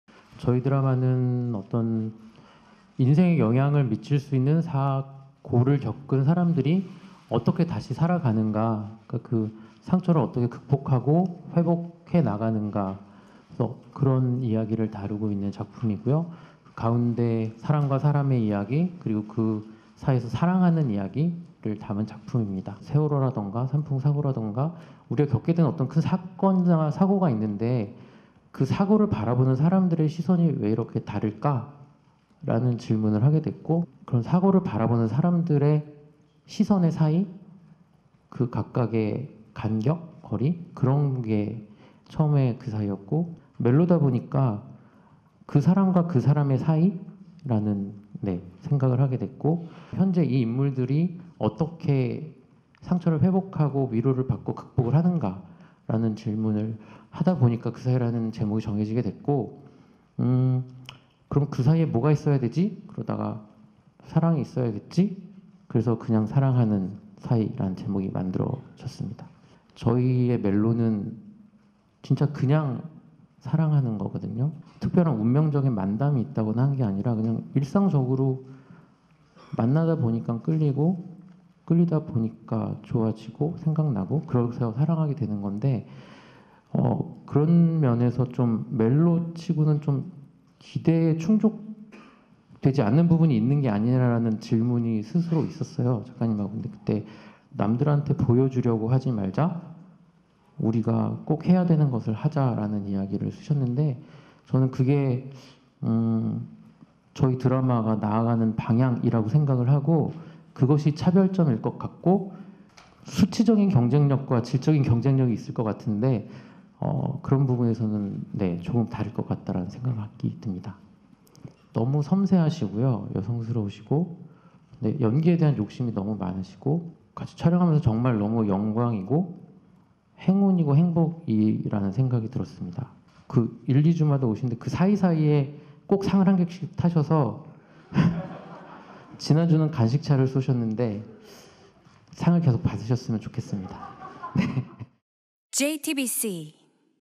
김진원 감독 "'그사이'는 일상적인 이야기, 그게 차별화"
큰 사고를 겪은 후 그 상처를 안고 살아가는 사람들.
그들의 일상과 사랑 이야기를 담은 [그냥 사랑하는 사이]

주변에서 일어날 수 있는 일상적인 이야기

[그냥 사랑하는 사이]
12월 11일 월요일 밤 11시 첫 방송!